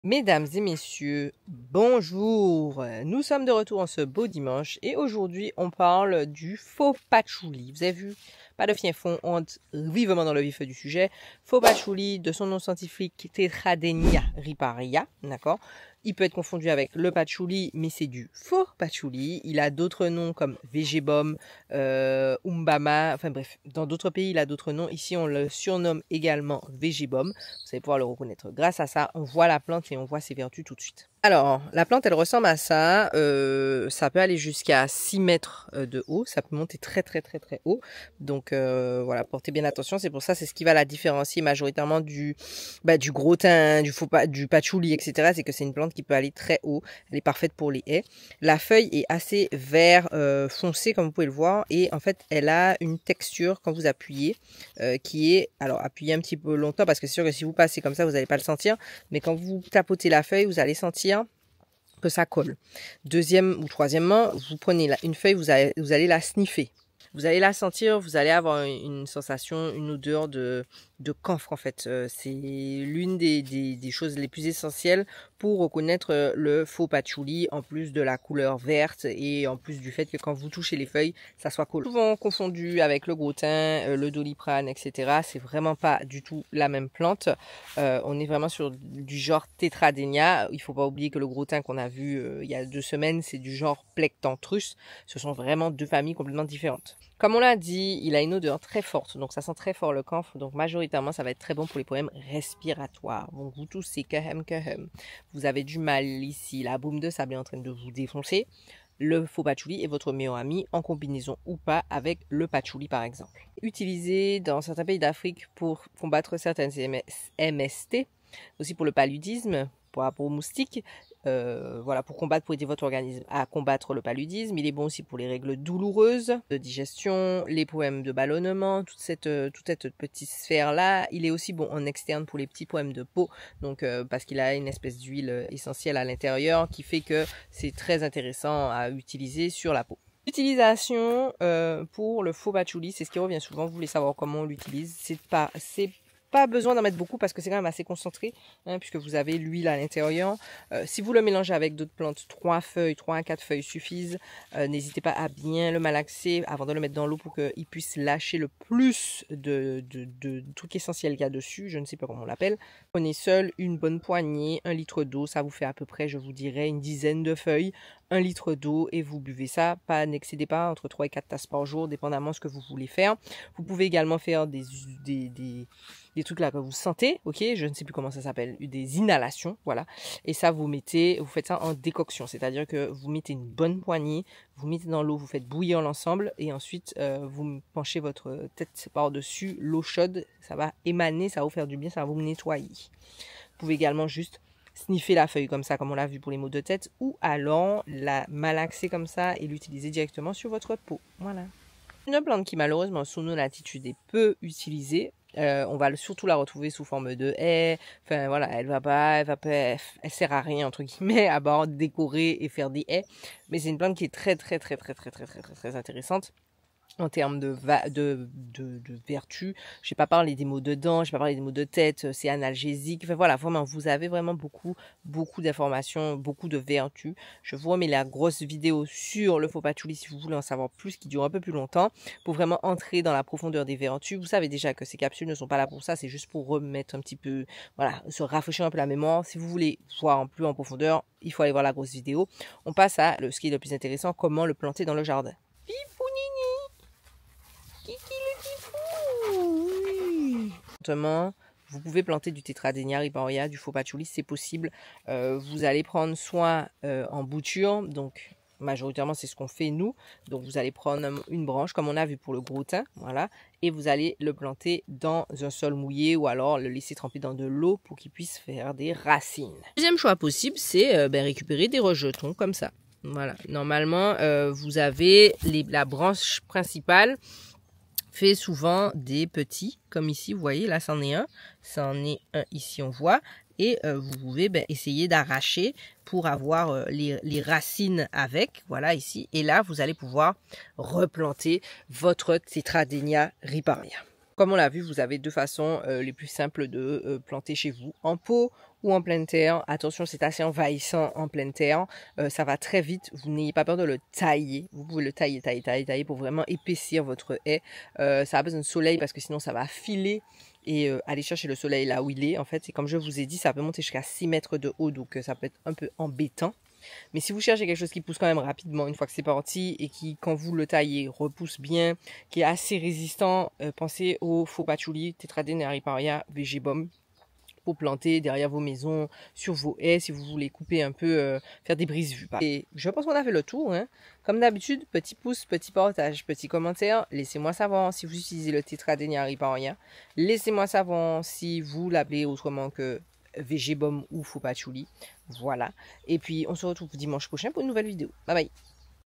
Les Bienfaits Cachés du Faux Patchouli (Tetradenia Riparia)
🌿 Découvrez le Faux Patchouli (Tetradenia riparia) 🌿
Le faux patchouli, ou Tetradenia riparia, est une plante aux multiples usages. Traditionnellement, elle est utilisée pour ses propriétés anti-inflammatoires et antibactériennes. En infusion ou en décoction, elle est idéale pour soulager les troubles respiratoires et les inflammations. C’est une plante incontournable pour ceux qui recherchent des solutions naturelles pour leur bien-être ! 💚

👍 Si vous avez aimé cette vidéo, n’hésitez pas à la liker et à la partager !